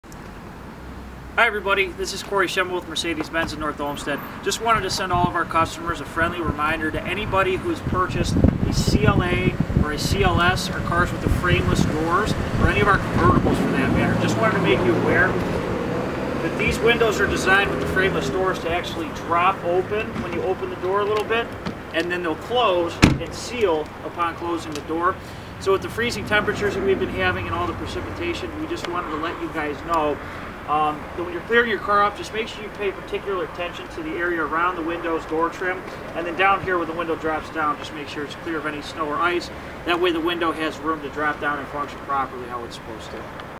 Living with Mercedes-Benz Frameless doors in the Winter.
We want to remind our Mercedes-Benz families out there to keep an eye on their frameless windows when freezing and icing can occur. found on the CLA, CLS, S-Coupe, SL roadster, SLK, and E-Class Cabriolets and Coupes!

Although there has been no reports or issues, we always want to help as much as possible!

-The Mercedes-Benz of North Olmsted Team!